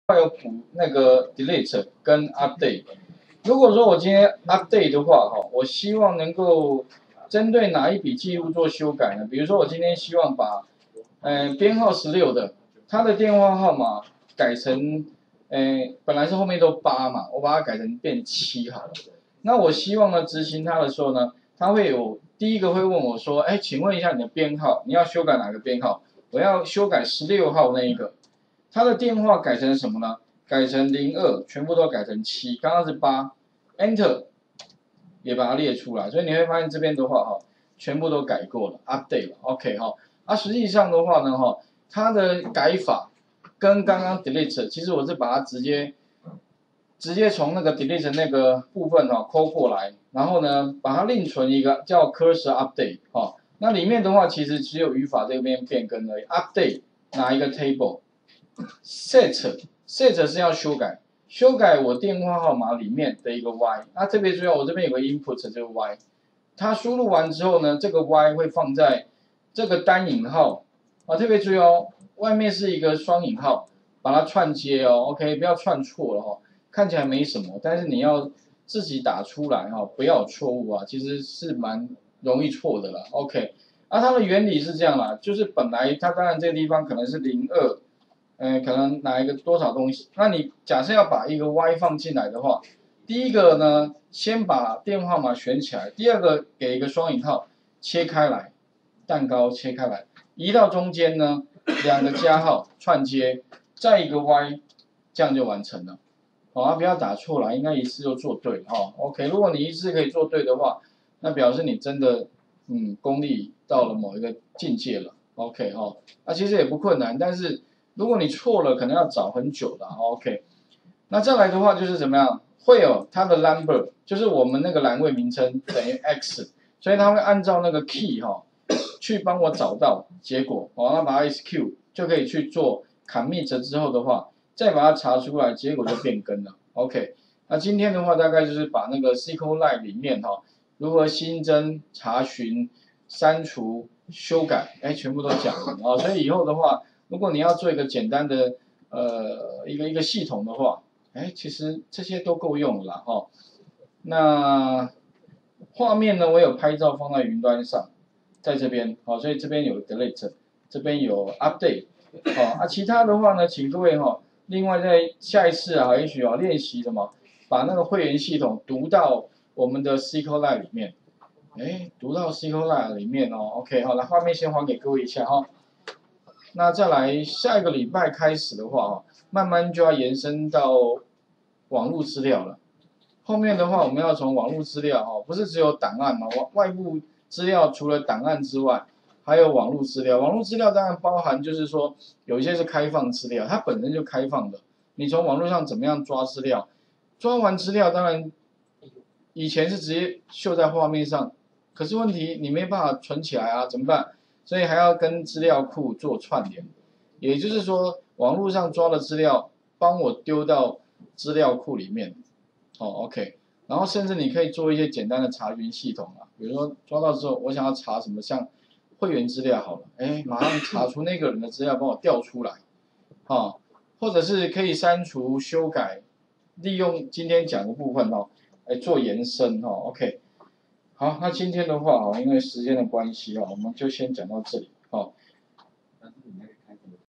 08 範例cursor update修改紀錄
從EXCEL VBA自動化到Python程式開發入門第6次上課(成績輸入改為檔案讀取複習&讀取member文字檔案與改為絕對路徑&將會員資料字串轉串列與只輸出其中兩欄&讀取CSV檔案&將MONTHCAI改為外部讀取與寫入文字檔&用字典輸入成績&變化題MONTHCAI改為字典與中翻英)

上課內容：
01_重點回顧與os套件相關方法說明
02_用walk方法列舉所有檔案與合併
03_合併程式除錯與除厝模式使用說明
04_範例cursor01建立資料庫與資料表並開啟
05_範例cursor02自動增號與輸入電話
06_範例cursor_select查詢所有資料表資料
07_範例cursor_delete刪除某筆紀錄
08_範例cursor_update修改紀錄

檔案處理
資料庫處理

上課用書：
參考書目

作者：葉難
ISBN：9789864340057
出版社：博碩文化
出版日期：2015/04/02

吳老師  106/9/9